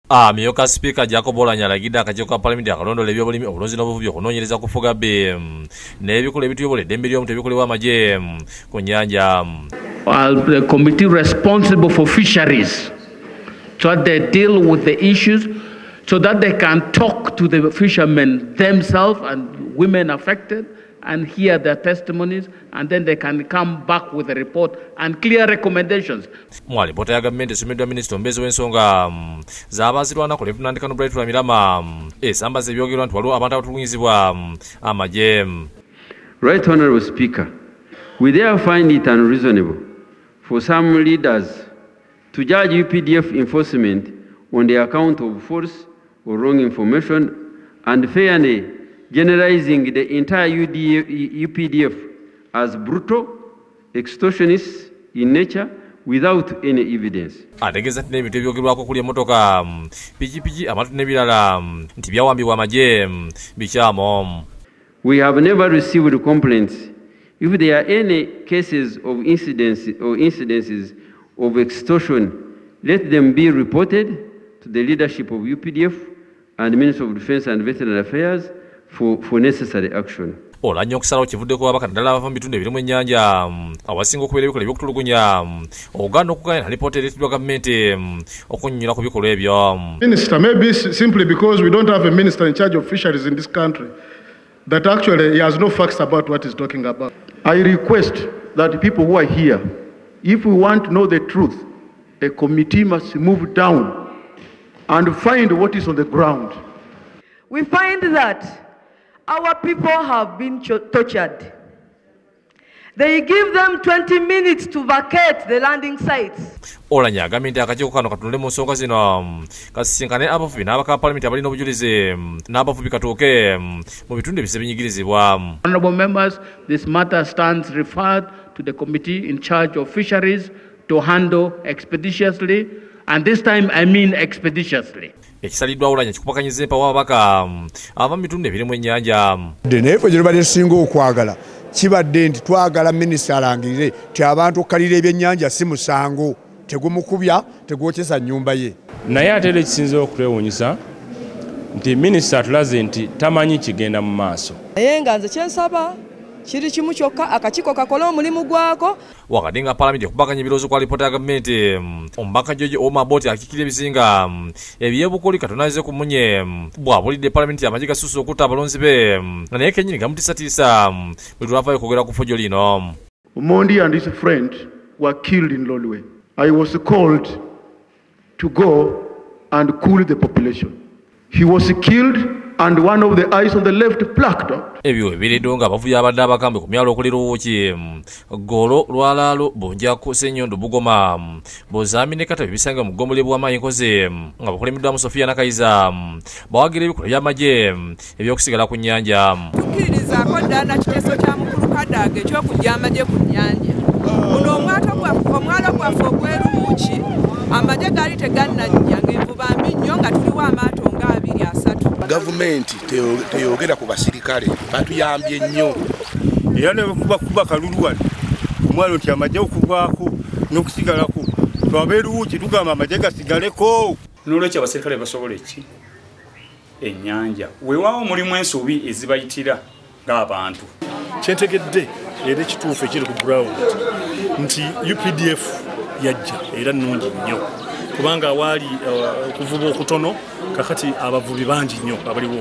Eby'okutulugunya abavubi tebinnaggwa.
Amyuka Sipiika alagidde akakiiko kanoonyereza.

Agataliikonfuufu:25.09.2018
Reporter: Obedi Lutale ne Racheal Nakasi